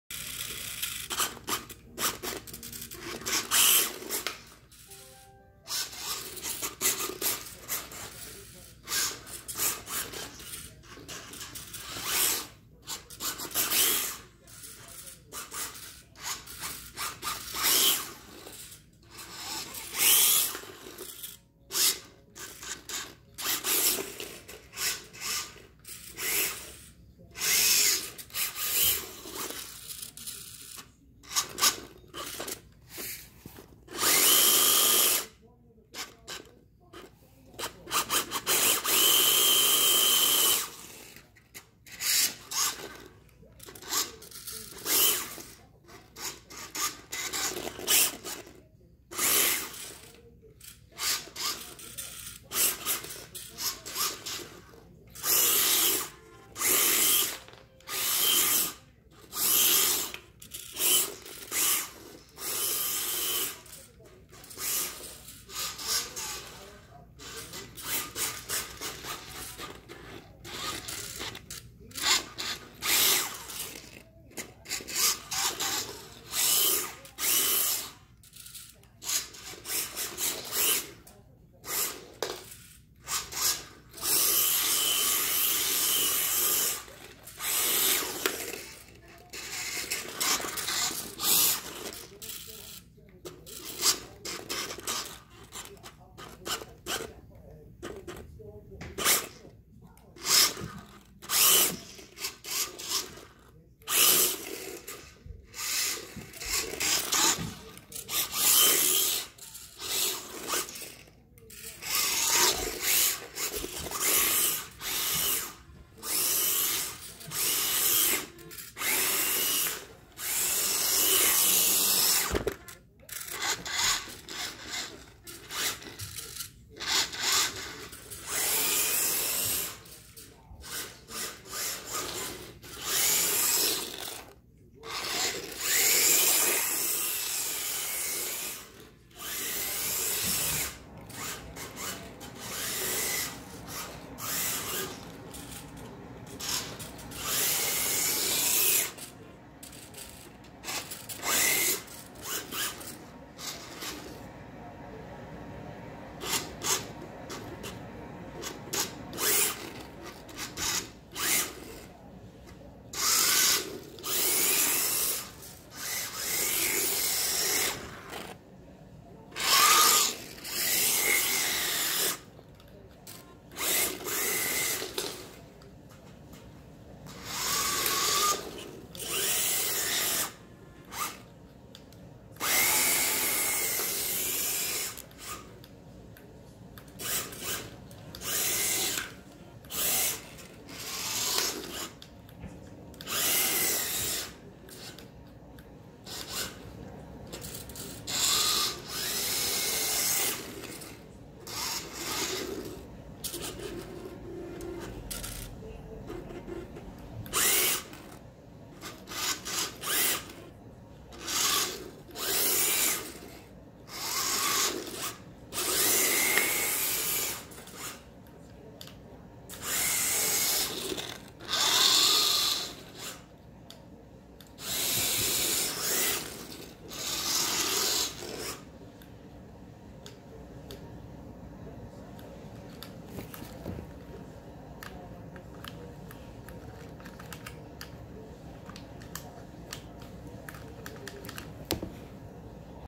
X mods rc slammed cutlass supreme
xmods rc with a 7.4 volt battery, chassis extended, body swap 80s cutlass supreme 442 slammed